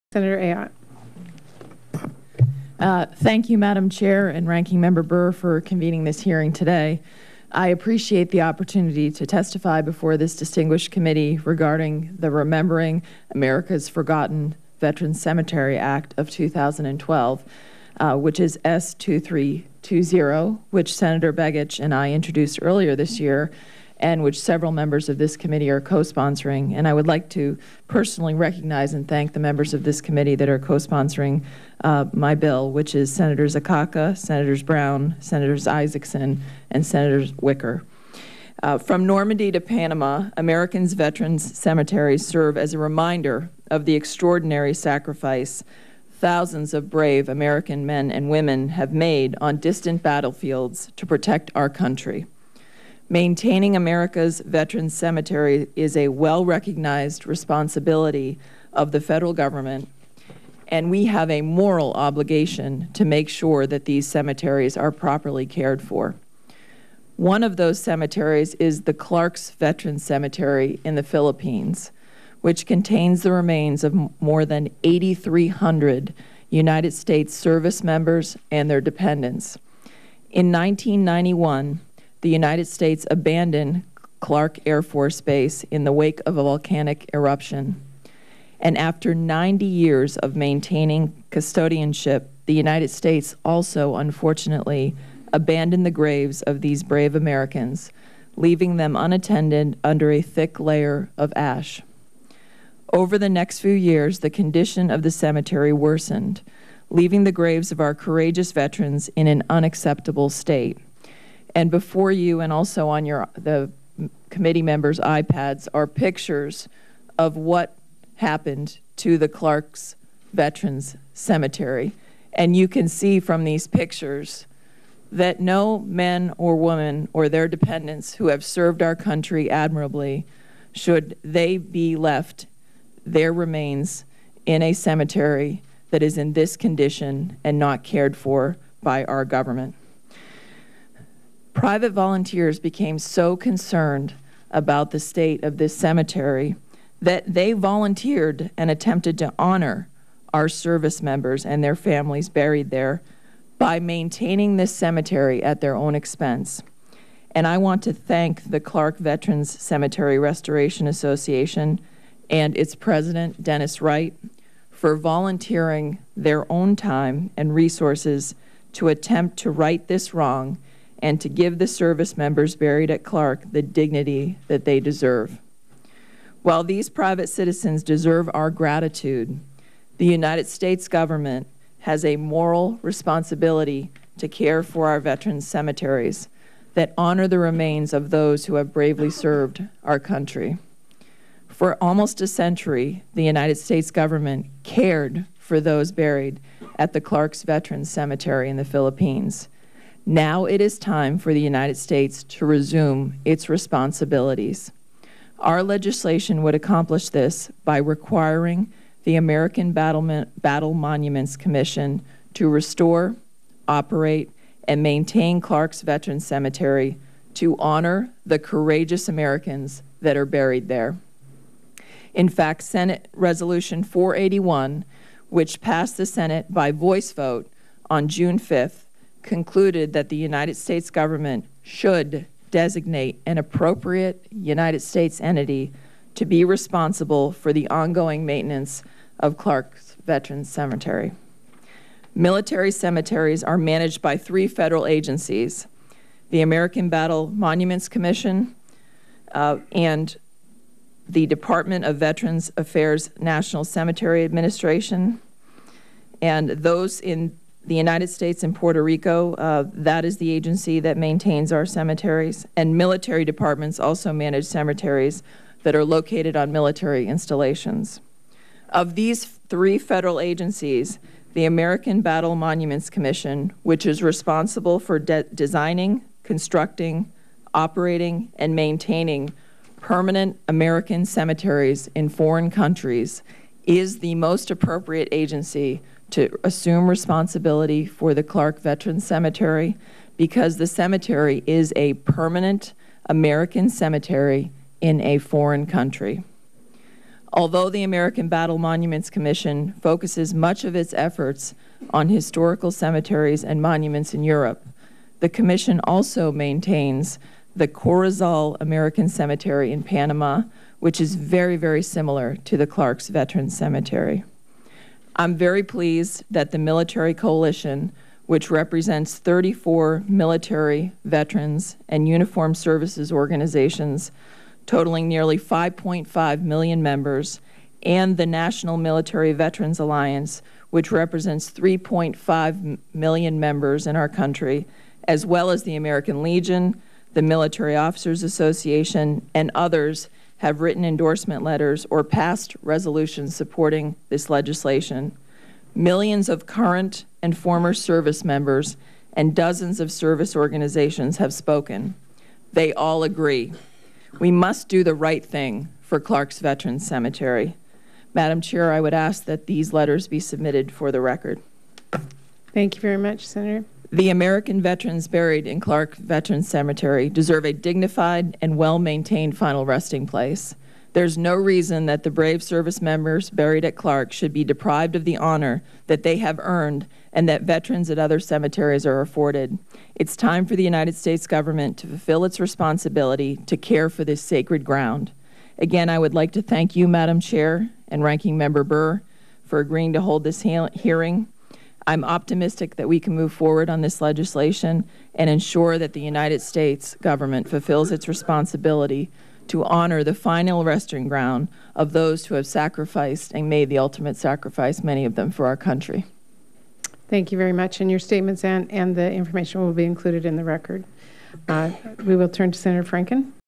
Ayotte Testifies In Support of Legislation Aimed at Restoring Clark Veterans Cemetery
U.S. Senator Kelly Ayotte (R-NH), a member of the Senate Armed Services Committee, testified today before the Senate Veterans' Affairs Committee in support of the Remembering America's Forgotten Veterans Cemetery Act.  The bill would require the U.S. to restore and maintain the Clark Veterans Cemetery in the Philippines which is the final resting place for more than 8,300 American service members and their dependents.  Following a volcanic eruption in 1991, the U.S. abandoned Clark Air Force Base, leaving the cemetery covered in ash and overgrown by weeds.